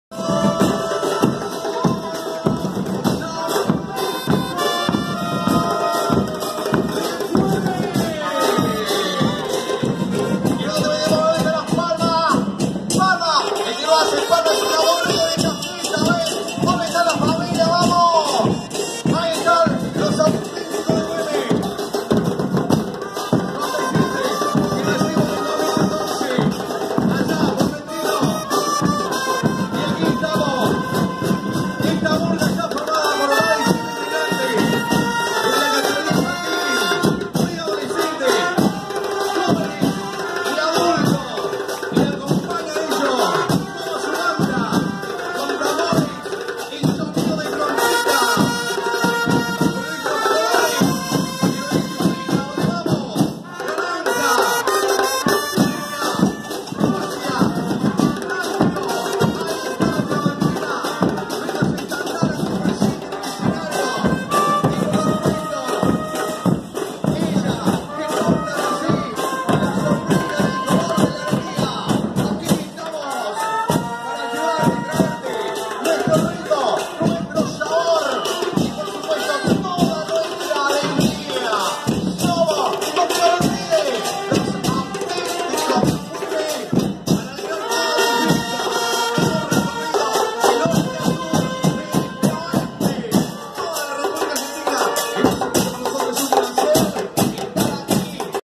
Centro Murga Los Autenticos de Güemes Pre-Carnaval 2020 parte3
los autenticos de güemes hacen su presentación en los pre-carnavales de lugano